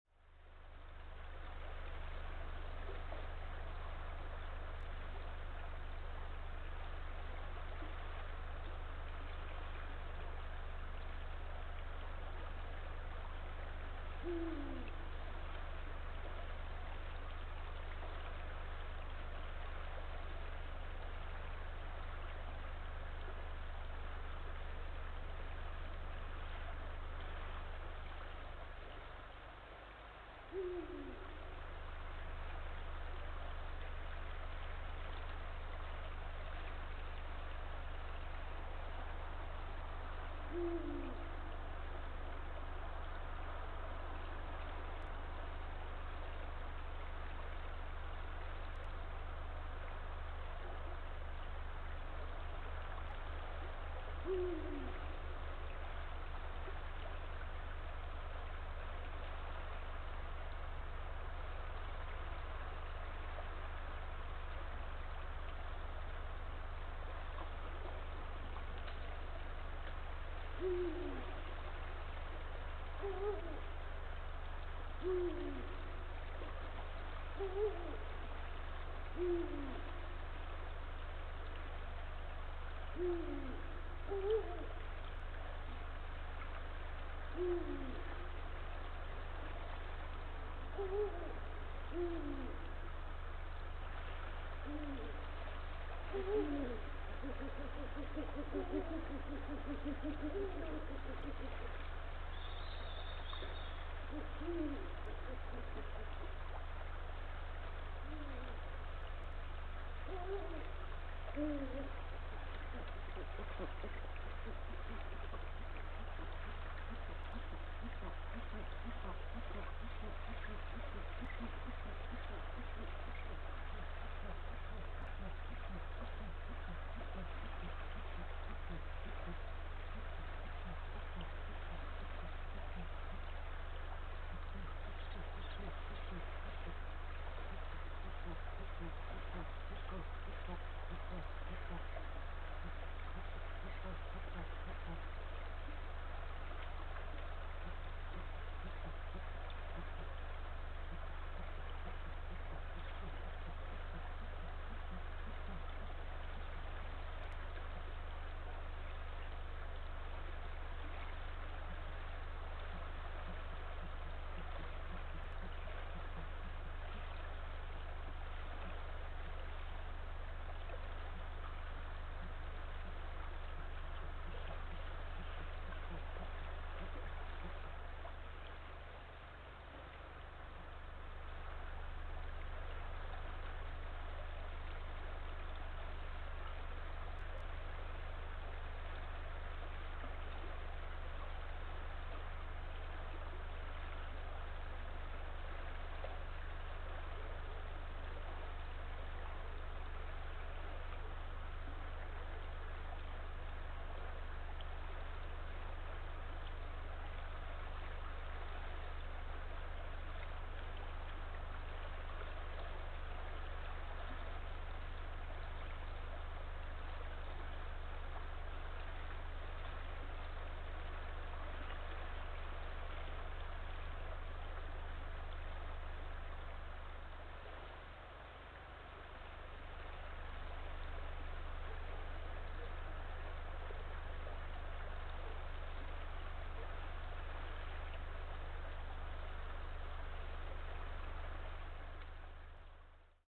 Nest Activity Close Up 🦉 03/21/23 18:30
The female Eagle Owl had spent an hour at her nest when she hears her mate call to her. She leaves the nest to join him off-cam. A series of hoots and grunts break out before copulation is heard. Then the male arrives at the nest, pecks around a bit, and departs.
The female arrived at 17:31 and Departed at 18:32.
The male arrived at 18:33 and Departed at 18:36.

Courtesy Of:
The Eagle Club Estonia’s YouTube channel
Supported by: The Estonian Fund for Nature toel.

Location: The northern coast of Estonia.